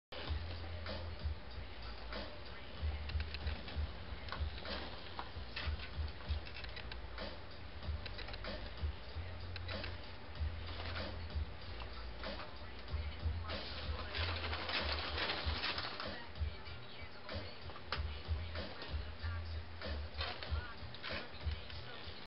Lawnmower_Shell_Test.wmv
Short test of the shell for my remote controlled lawn mower.